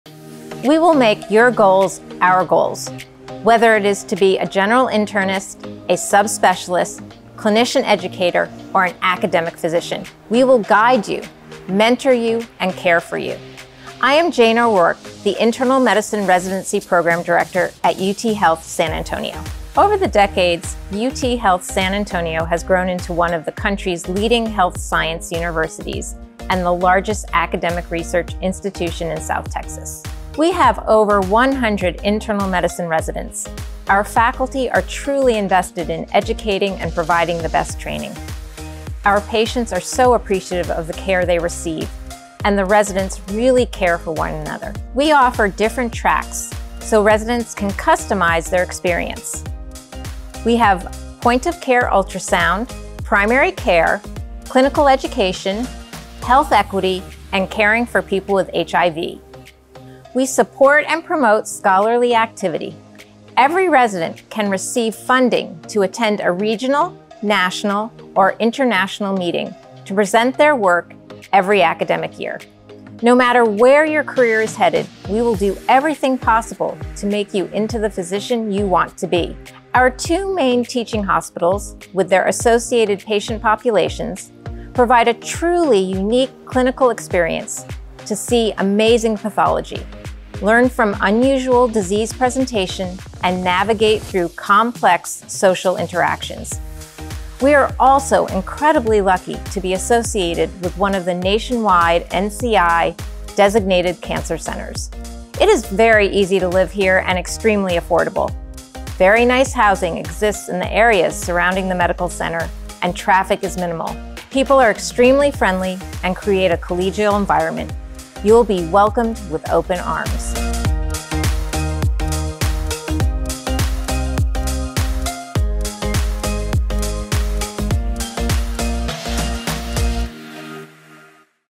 The Internal Medicine Residency Program at UT Health San Antonio Long School of Medicine
The Internal Medicine Residency Program at UT Health San Antonio, Texas, was established in 1970, only a few years after the land, a dairy farm, was conveyed to the State of Texas to build a medical school. Over the decades, it has grown into one of the country’s leading health science universities and the largest academic research institution in South Texas. The U.S. Department of Education has designated us as a Hispanic-serving Institution, and we consistently rank among the top schools in the nation for graduating Hispanic health care providers. UT Health San Antonio and the Internal Medicine Residency strive to provide the best in residency education, patient care, biomedical research opportunities and community service.